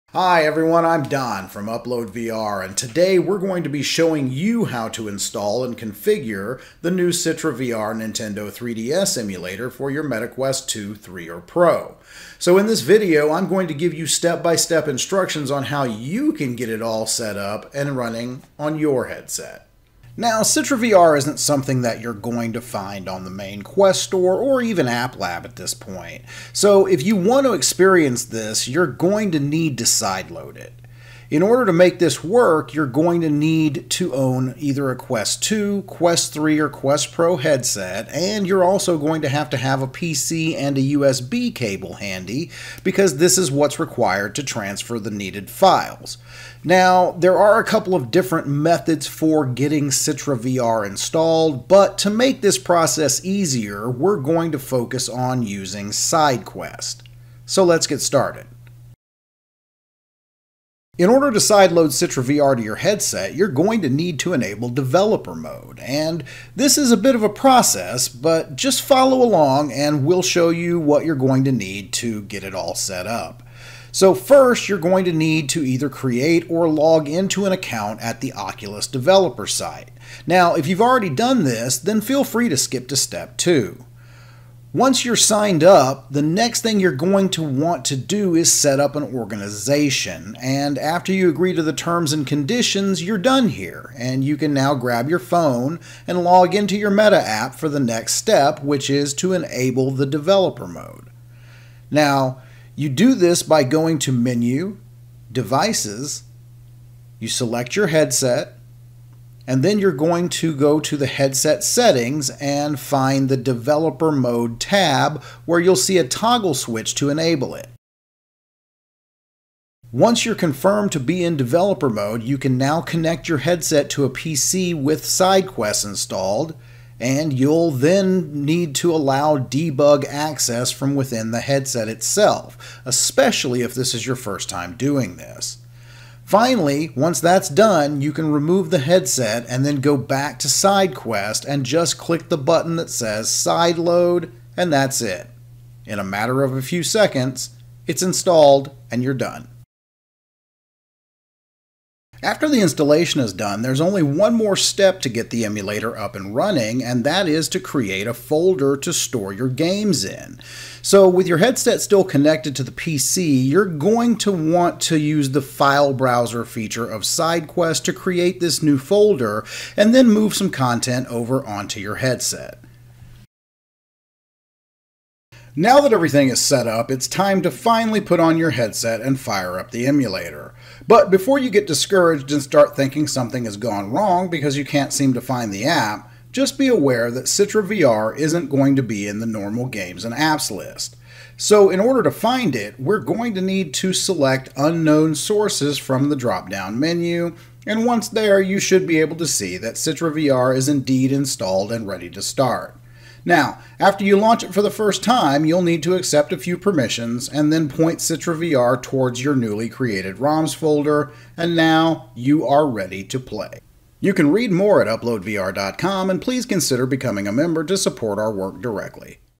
How To Install CitraVR Nintendo 3DS Emulator On Quest 2, 3, Pro
CitraVR is a Quest port of the leading Nintendo 3DS emulator Citra. The project from Amanda Watson can be found on Github and installed to Meta Quest 2, Quest 3, or Quest Pro via SideQuest:

0:00 Introduction
0:58 Step 1: Set up developer mode
1:57 Step 2: Sideload the emulator
2:29 Step 3: Create a ROMs folder
2:53 Step 4: Find and launch CitraVR

If you’re having trouble, check out our written guide wirh more details